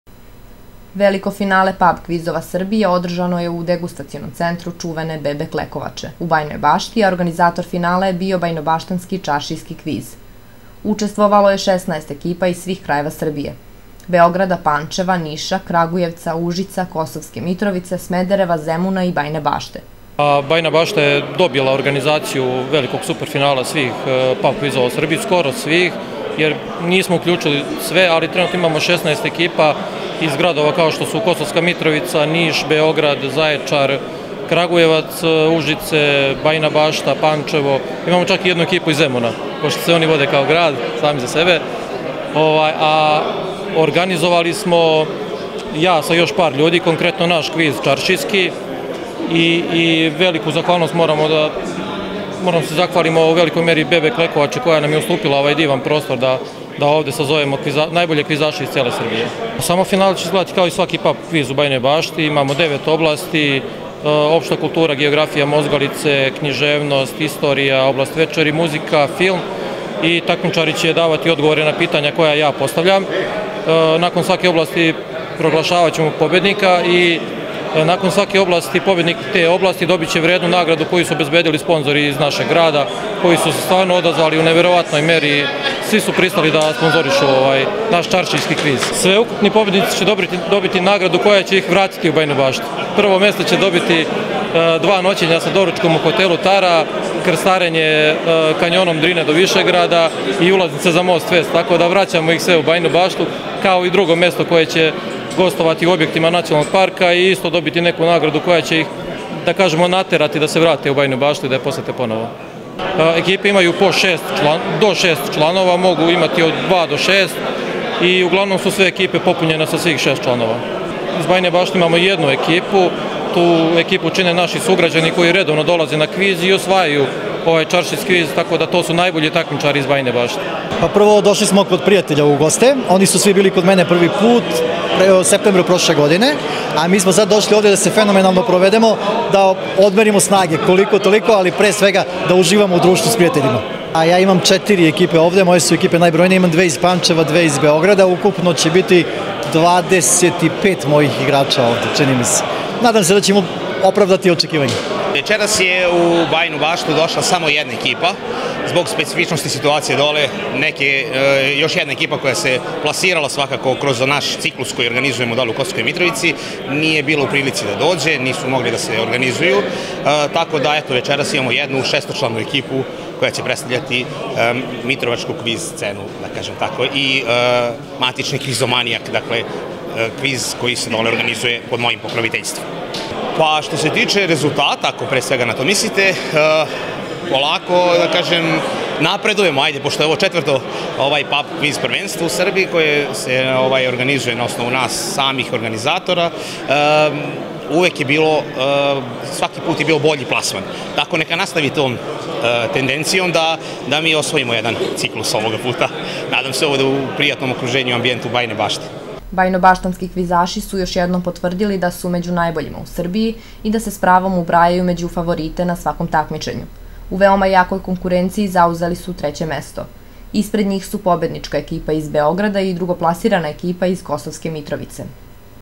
Finale PAB KVIZA
U Subotu je u Bajinoj Bašti održano veliko finale PAB KVIZ-a, uz učešće 16 najboljih ekipa sa preko 100 takmičara iz svih krajeva Srbije.
Domaćin ovogodišnjeg finala je Čaršijski kviz iz Bajine Bašte, a učesnici su imali priliku da svoje znanje pokažu na jednoj izuzetnoj lokaciji, u atraktivnom degustacionom centru jedne od najboljih srpskih destilerija – BB Klekovače.